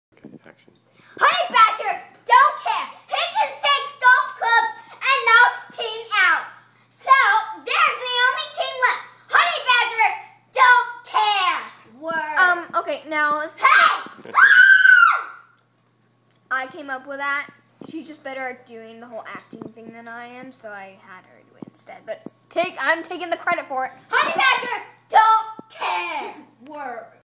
Lauren "honey badger" Burton
Elise choreograph's lauren's honey badger routine